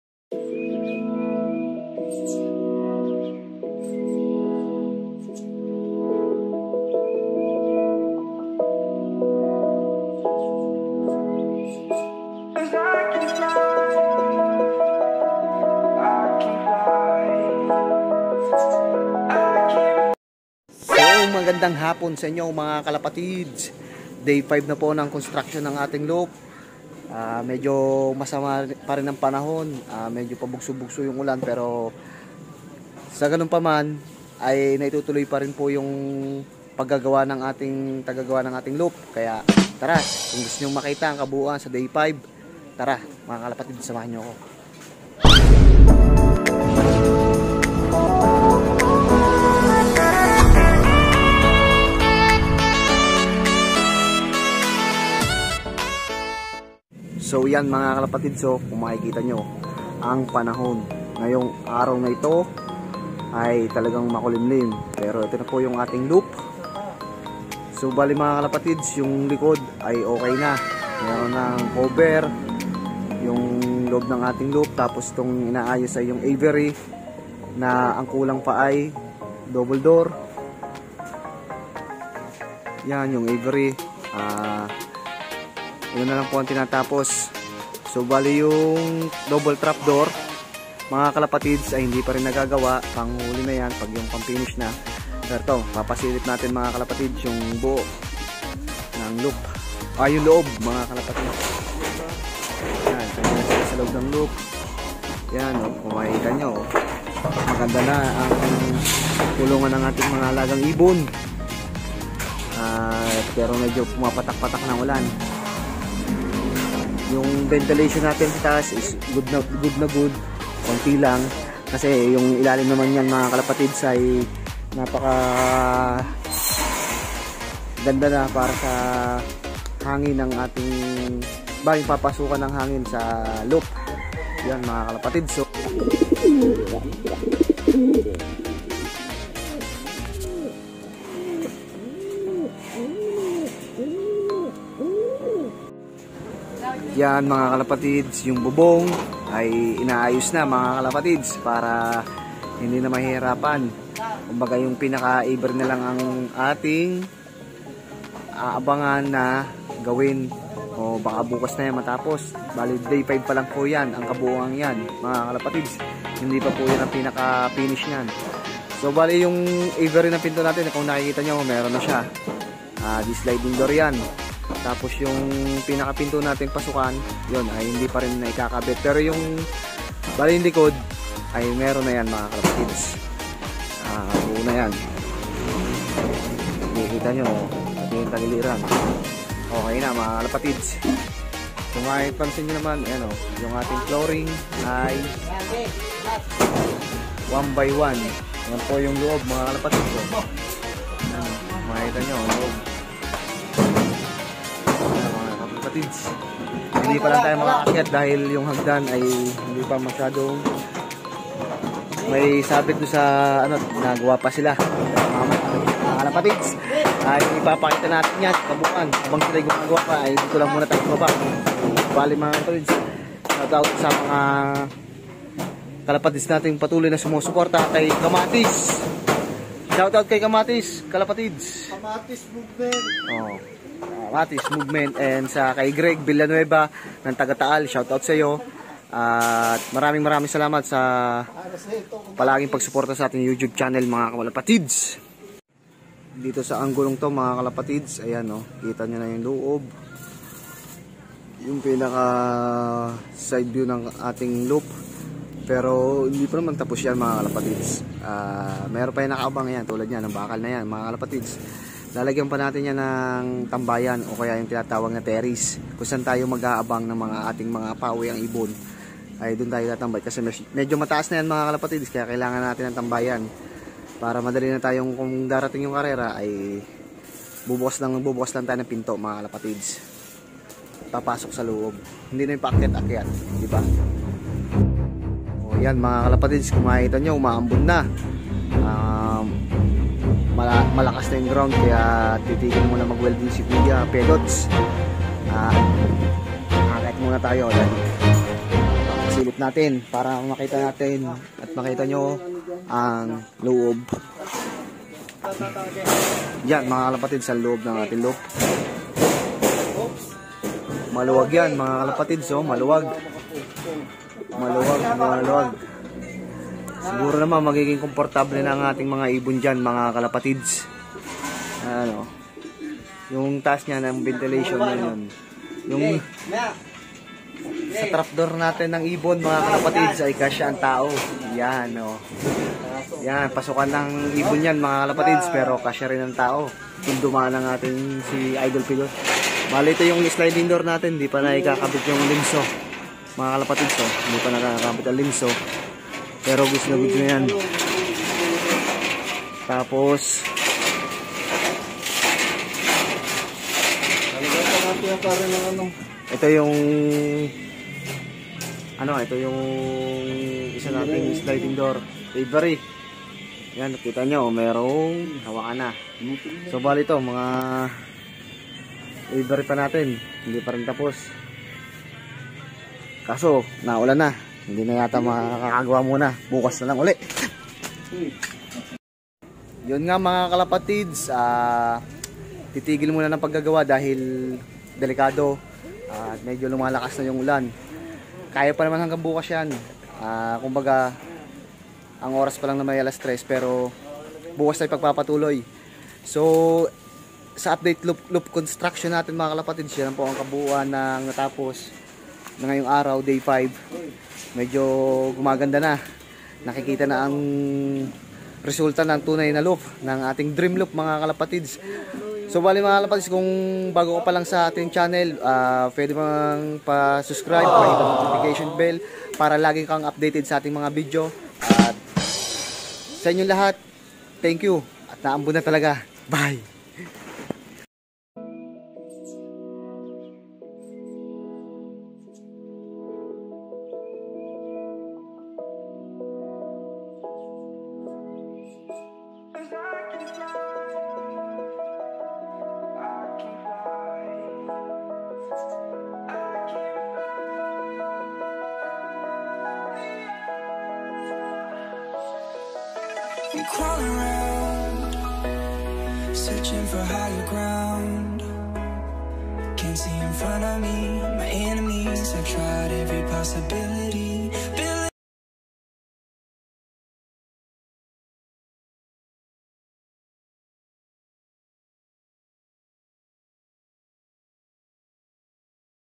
DREAM LOFT ❣️🏆 LOFT CONStruction Day 5 | Bayabas Loft
SALAMAT sa Mga Alaga kong Kalapati Dahil sa Inyo ay Nakapag Pagawa ako ng Loft At Sukli ko na din sa inyo kaya Sana Mas pag butihin pa natin sa Bawat Laban